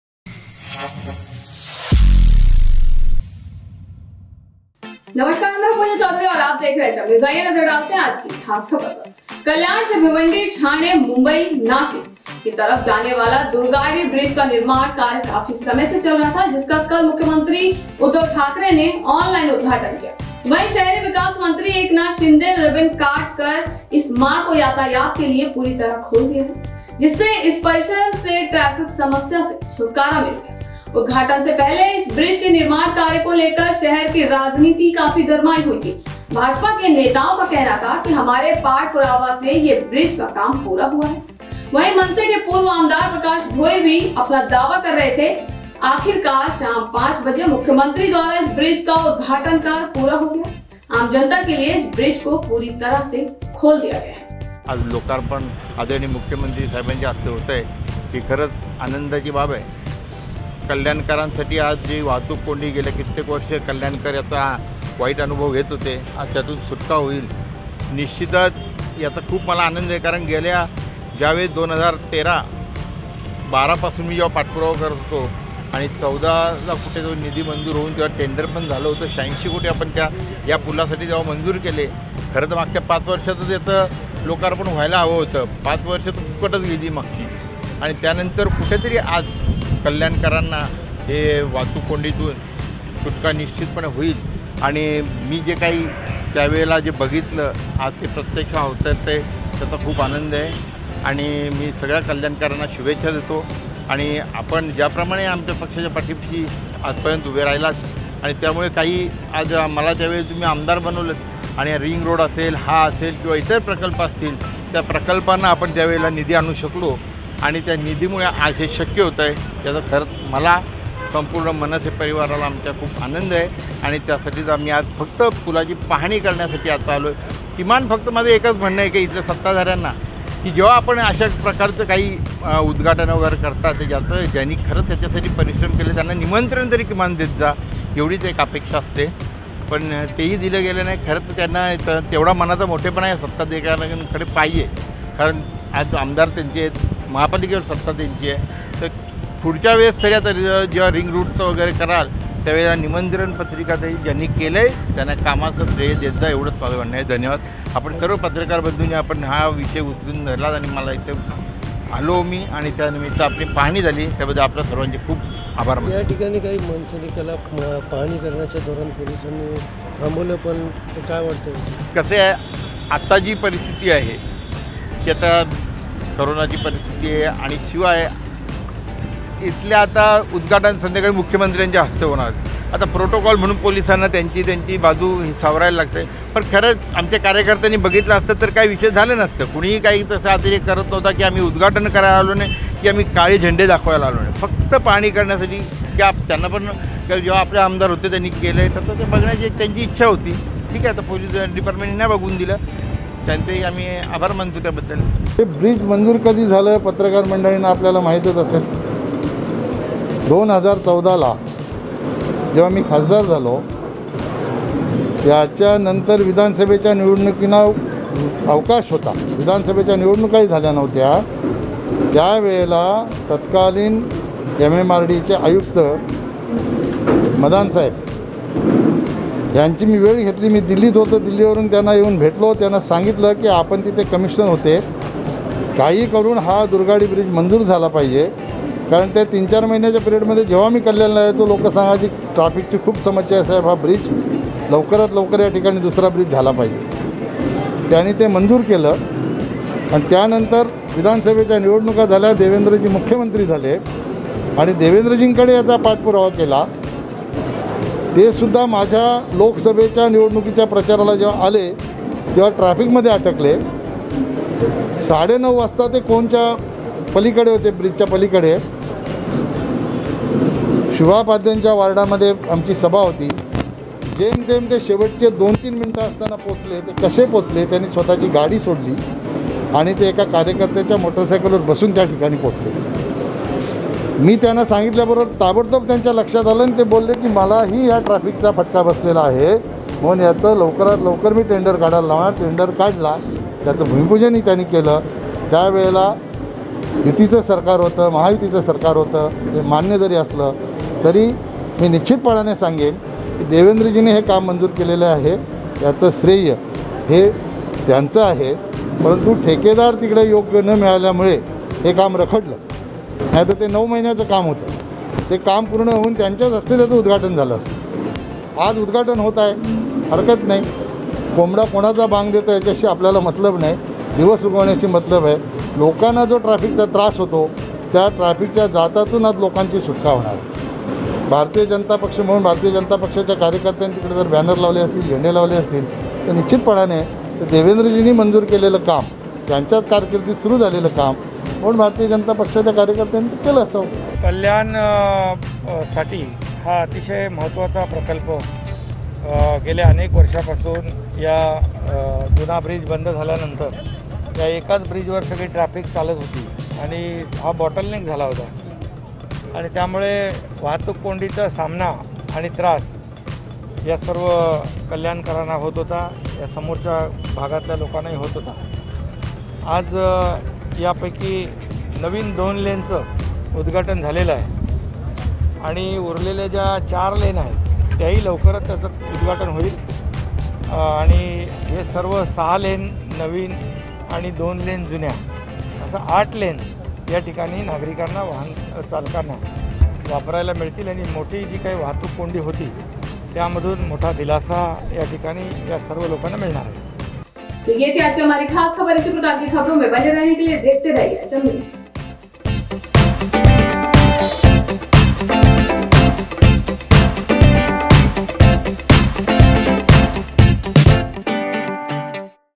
CM उद्धव ठाकरे ने किया ऑनलाइन दुर्गाडी ब्रिज का उद्घाटन | देखिये खास रिपोर्ट | HM News
कल्याण से भिवंडी, ठाणे ,मुंबई, नासिक , की तरफ जाने वाला दुर्गाडी ब्रिज का निर्माण कार्य काफी समय से चल रहा था जिसका कल मुख्यमंत्री उद्धव ठाकरे  ने ऑनलाइन उद्घाटन  कर दिया वही शहरी विकास मंत्री एकनाथ शिंदे रिबिन काटकर  इस मार्ग को यातायात के लिए पूरी तरह खोल दिये
जिससे इस परिसर की ट्रैफिक समस्या से  छुटकारा मिल गया उद्घाटन से पहले इस ब्रिज के निर्माण कार्य को लेकर शहर की राजनीति काफी गरमाई हुई थी भाजपा के नेताओ का कहना था की हमारे    पाठ पुरावा से यह ब्रिज का काम पूर्ण हुवा वहीं  मनसे के पूर्व आमदार प्रकाश भोईर भी अपना दावा कर रहे थे
आखिरकार साम 5 बजे मुख्यमंत्री द्वारा इस ब्रिज का उद्घाटन कार्य पूरा हो गया आम जनता के लिए इस ब्रिज को पूरी तरह खोल दिया गया।
The construction work of Durgadi Bridge from Kalyan to Bhiwandi, Thane, Mumbai, Nashik, was going on for a long time, which was yesterday inaugurated online by Chief Minister Uddhav Thackeray, while Urban Development Minister Eknath Shinde Ribin cut this road to complete the traffic. kind of opened
Due to which the traffic problem of this complex got rid of. Before the inauguration, there was a lot of heat in the politics of the city regarding the construction work of this bridge. BJP leaders said that the work of this bridge was completed with our lesson Purva, while the former general of MNS Prakash. Bhoir was also claiming
Finally, the inauguration work of this bridge was completed by the Chief Minister at 5 o'clock. This bridge was fully opened to the general public.